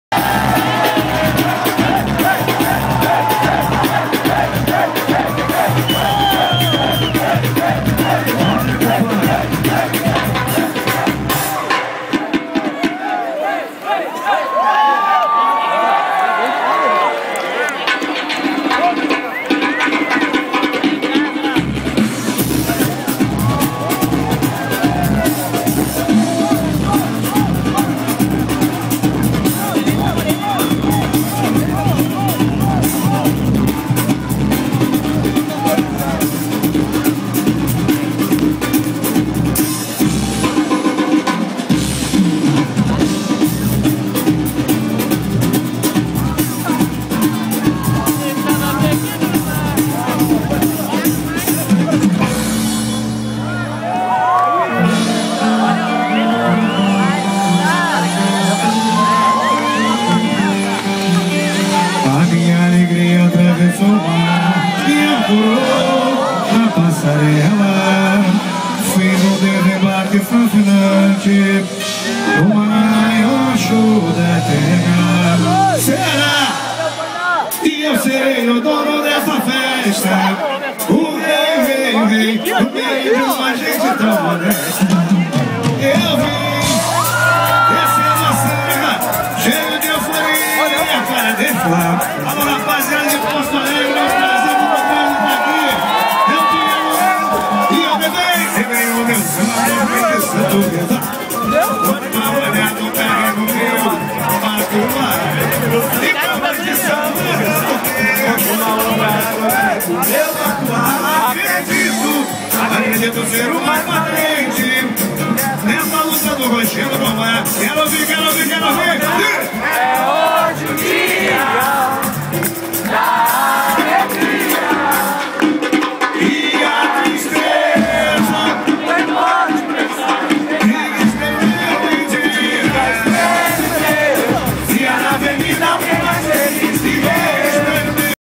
Dudu Nobre na Fan Fest Porto Alegre
Show do Dudu Nobre na Fan Fest de PoA após o jogo Alemanha 2X1 Algeria pelas oitavas de final da Copa do Mundo 2014.